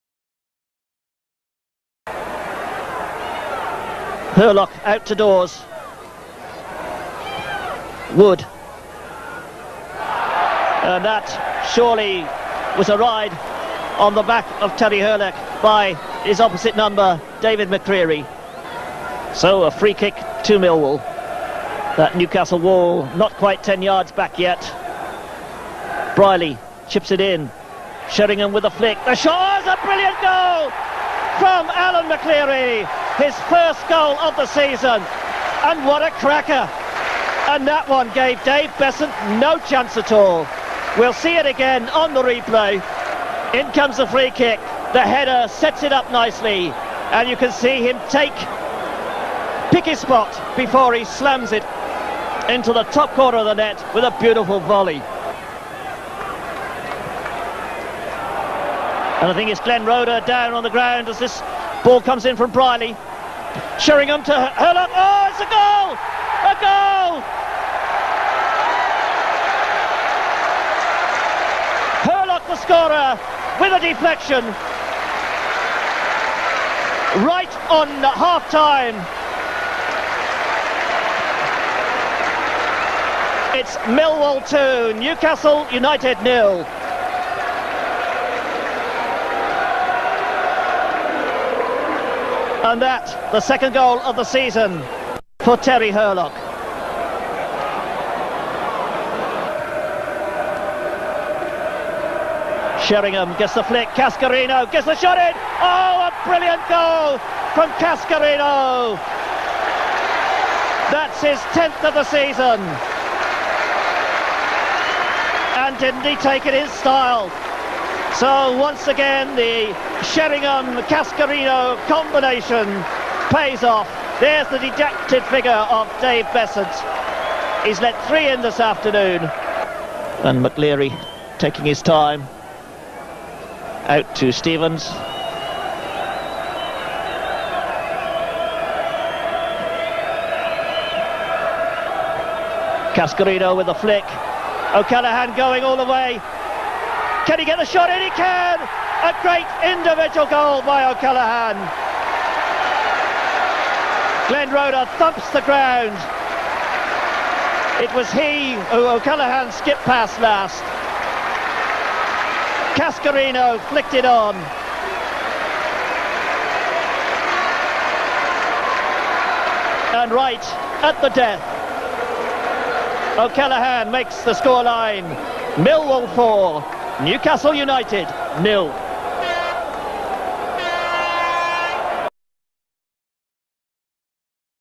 Millwall v Newcastle Utd 1988 89 Millwalls biggest win
Some clips of Millwalls biggest win in the two seasons they spent in the top Division played at the old Den